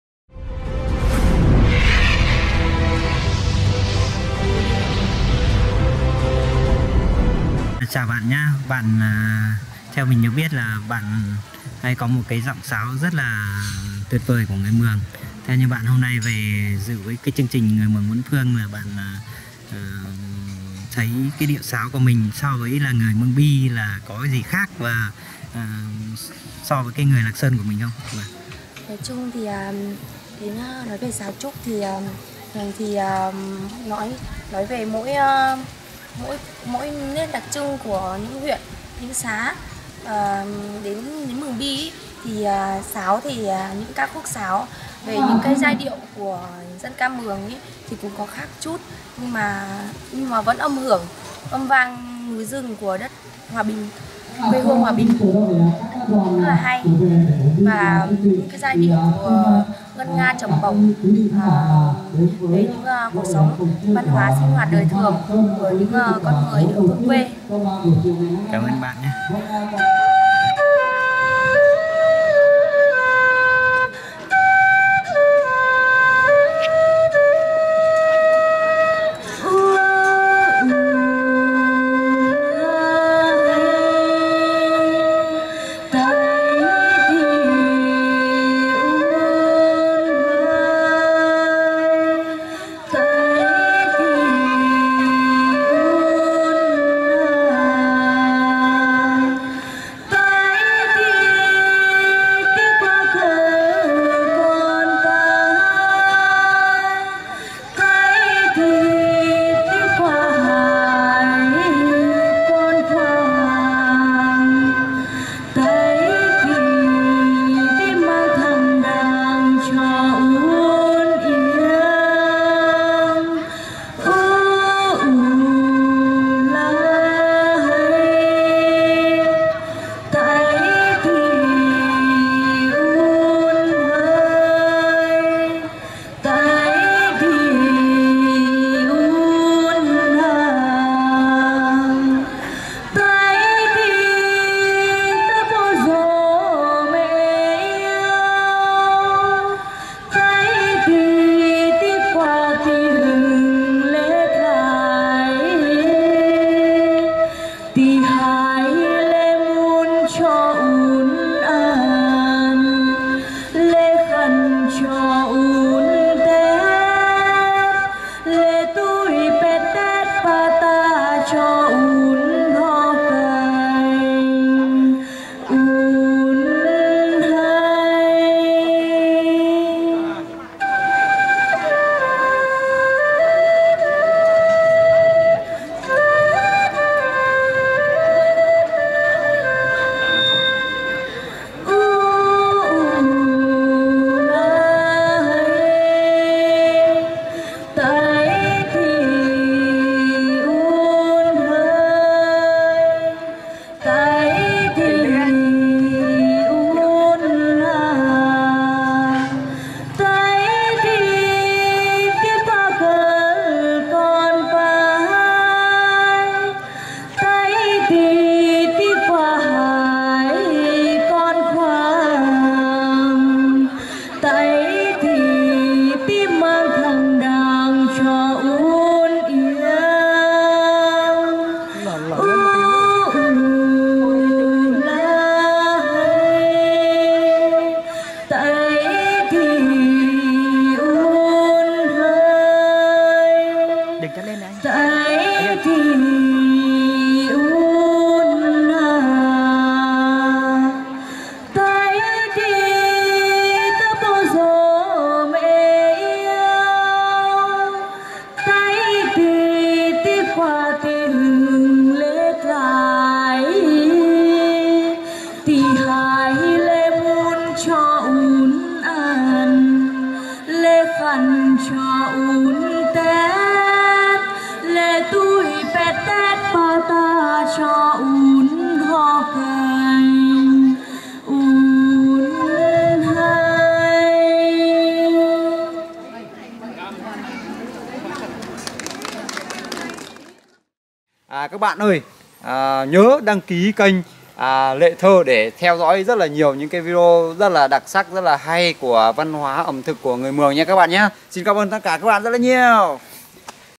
hai nghệ nhân trẻ tuổi nhất sứ mường hát ru ún
gặp mặt Người Mường Bốn Phương